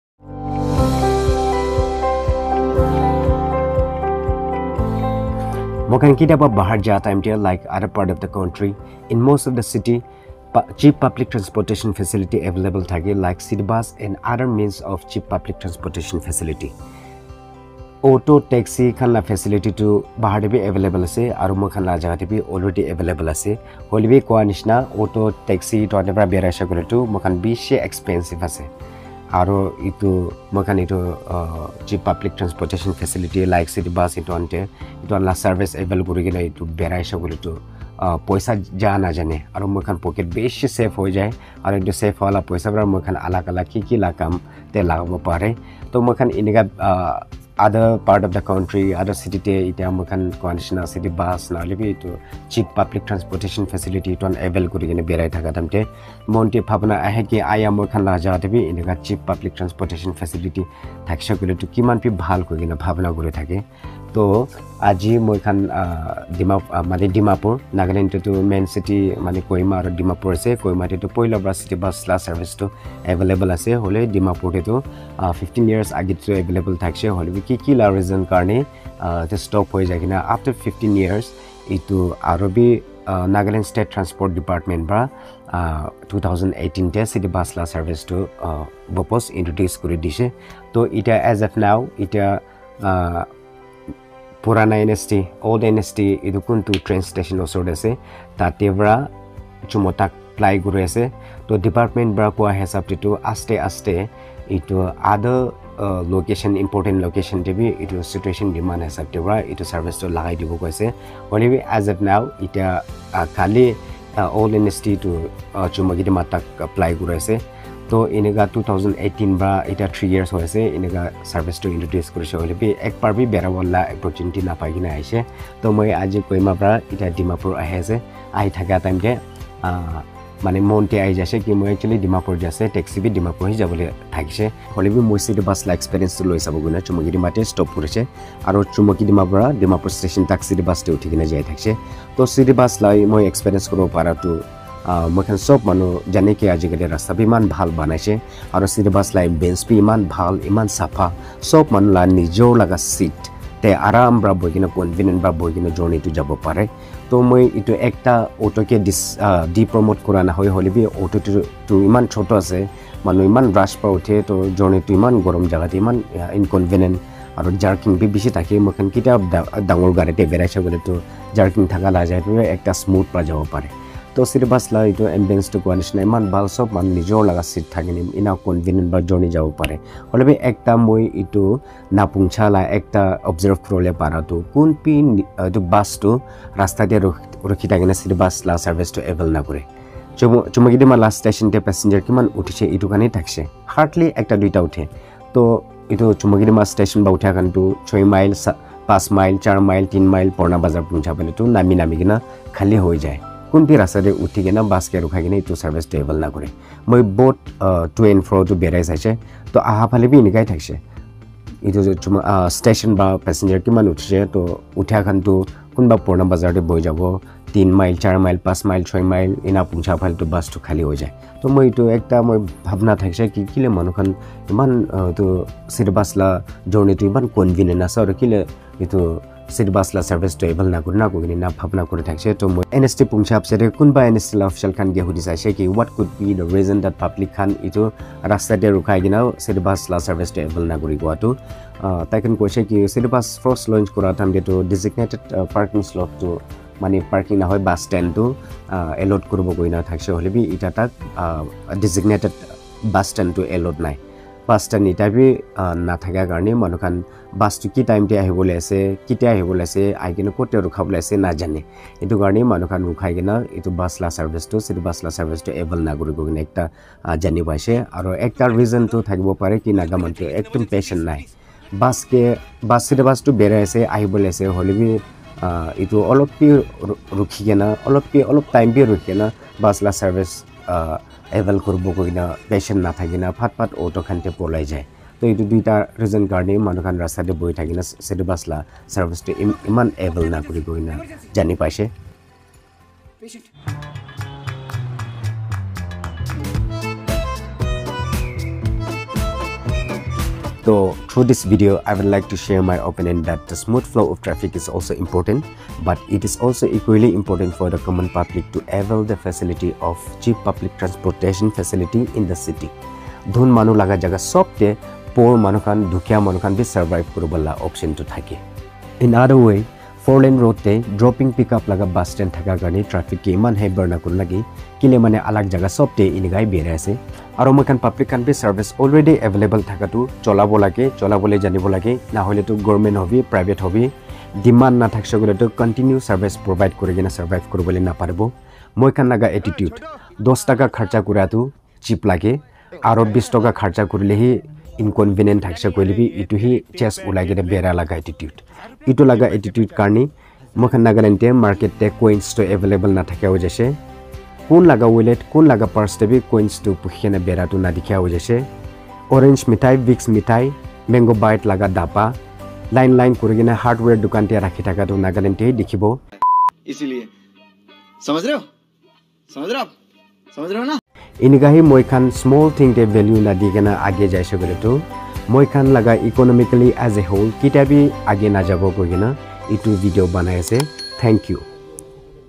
Value of Small Thing || Why it is Important to Make Use of Already Available Facility in Your City!?
VALUE OF SMALL THING

Points Covered:
1. Importance of Cheap public transportation facility in the City.
2. Why Public should make use of it.
3. Importance of Bus Stop where City Bus are in service for the safety, smooth traffic flow and for the convenient of citizen.
4. Value of Small Thing.